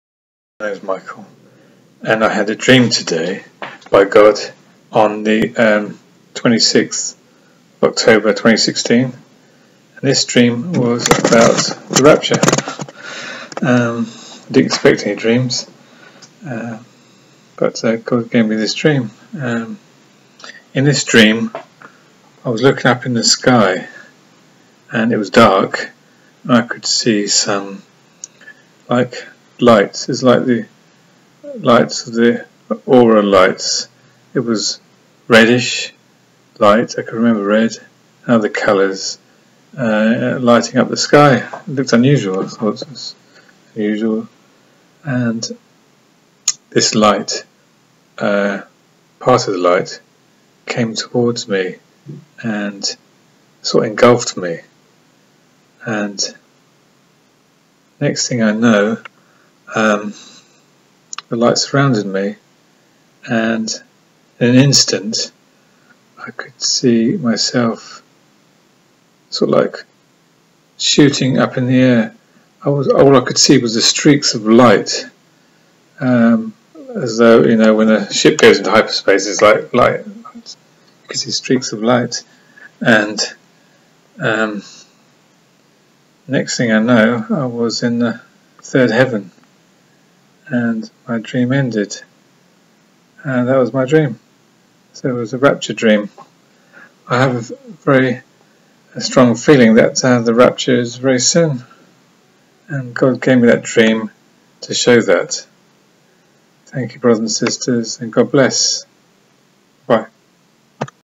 RAPTURE DREAM ( october 26th 2016 )
God gave me a rapture dream , and I believe God is telling me that the rapture is soon . it felt so real like I was taken in my sleep. God bless and look up our redemption draws near.